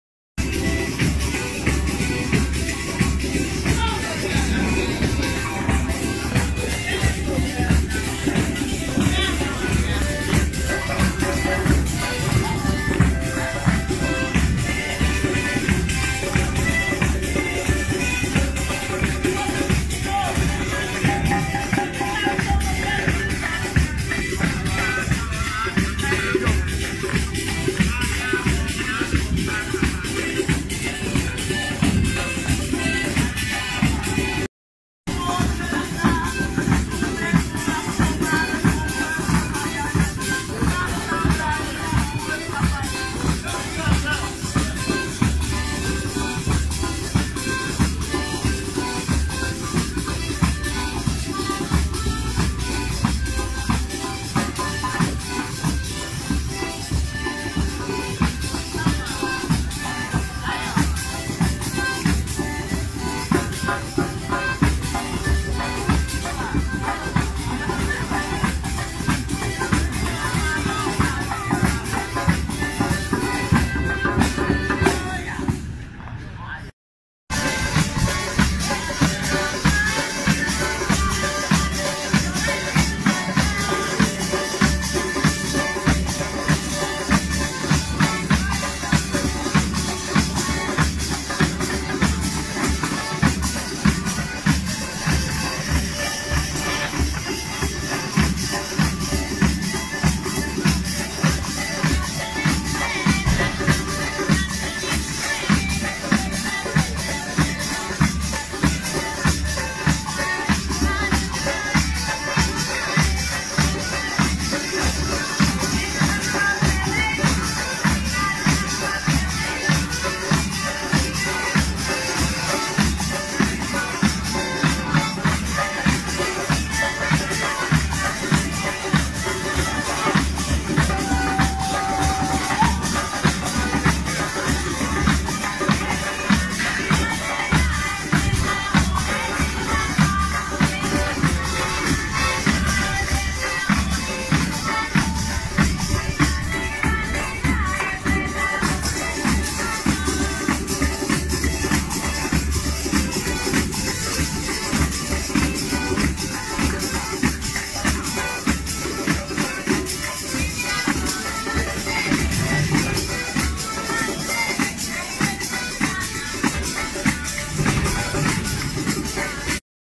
piazza cavallotti cutrofiano rosella salvucci romolo crudo
dopo il concertone degli ucci festival a cutrofiano -ronde fino alle tre del mattino